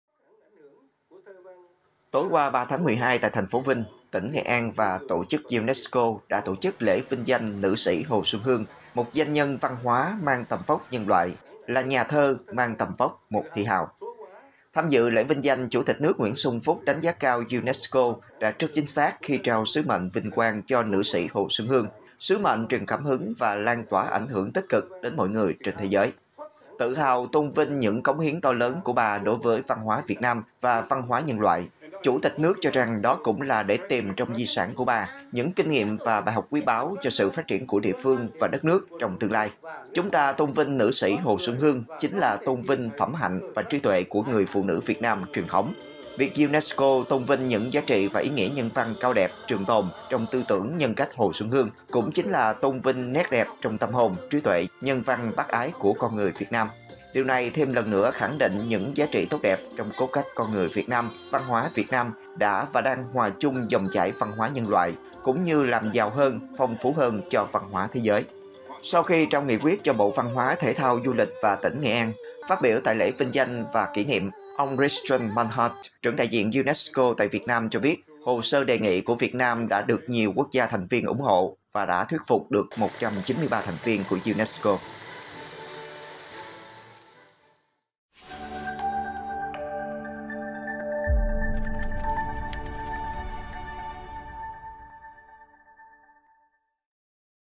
UNESCO vinh danh Nữ sĩ Hồ Xuân Hương
Tổ chức UNESCO vinh danh Nữ sĩ Hồ Xuân Hương - một Danh nhân văn hóa mang tầm vóc nhân loại, là nhà thơ mang tầm vóc một thi hào.